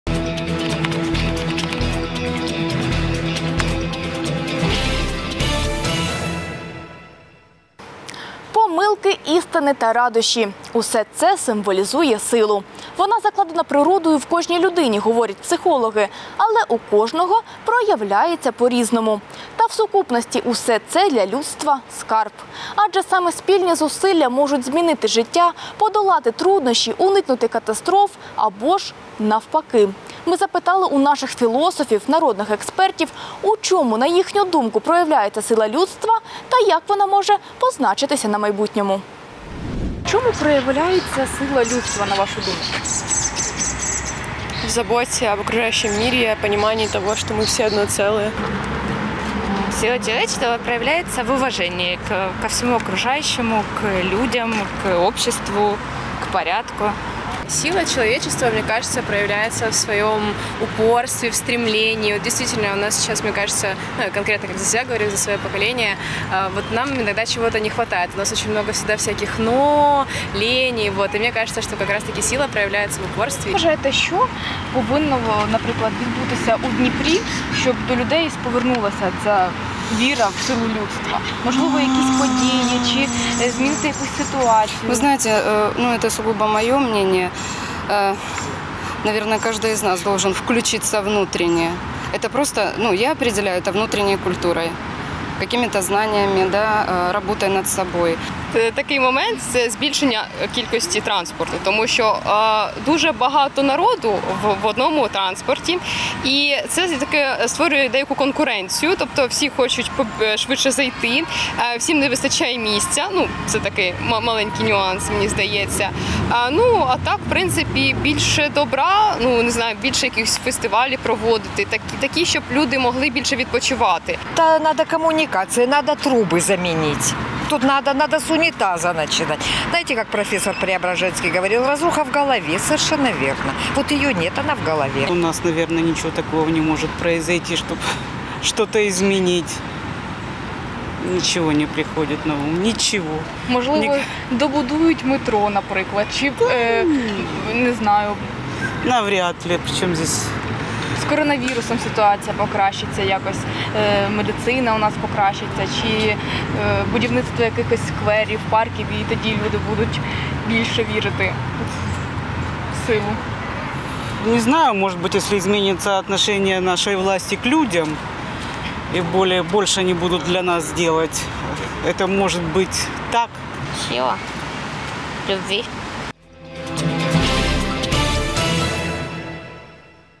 Сила людства
У чому проявляється сила людства?